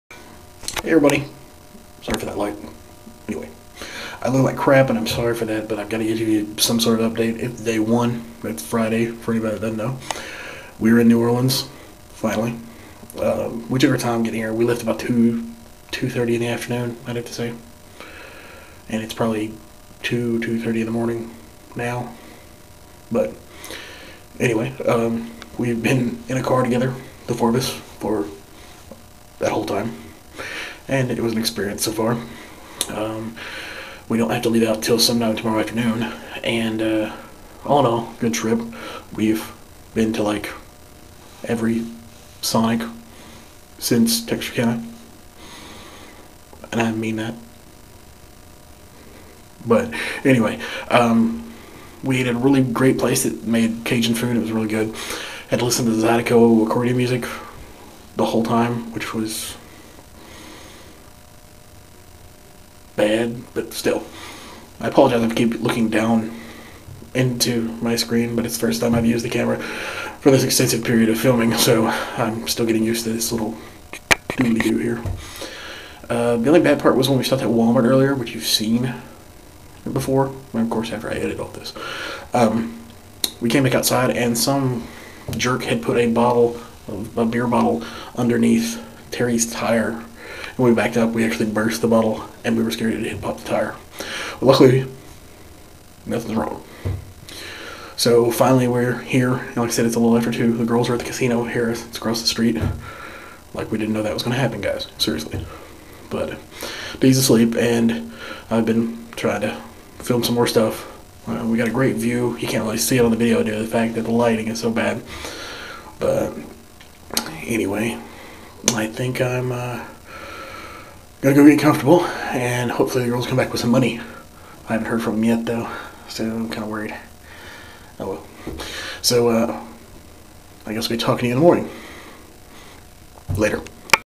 Trip Update!
Scotty updates on the cruise trip so far...